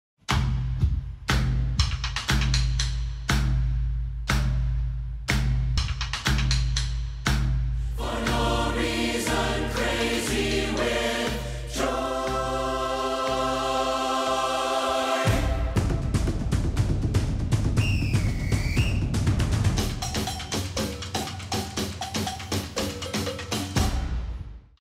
October Project Virtual Choir of Joy TRAILER
Featuring 163 singers, dancers, drummers and performers from 15 countries, 90s pop band and present day creative collaborative October Project presents their Virtual Choir of Joy  as part of an initiative to spread joy through music.